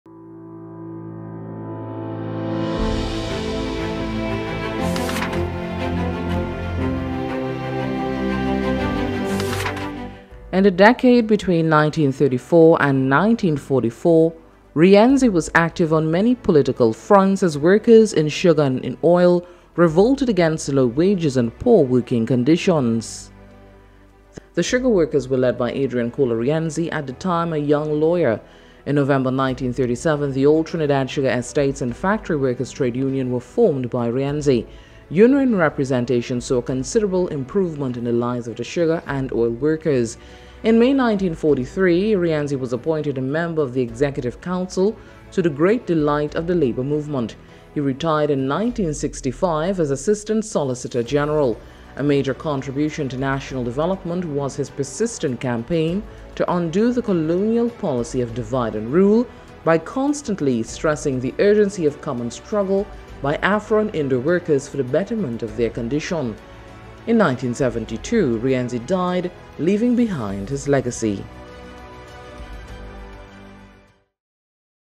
Did You Know? - Adrian Cola Rienzi Part 2
Legendary Unionist Adrian Cola Reinzi encountered many battles during his active years as a trade union leader, but that did not prevent him from fighting for the rights of sugar cane workers.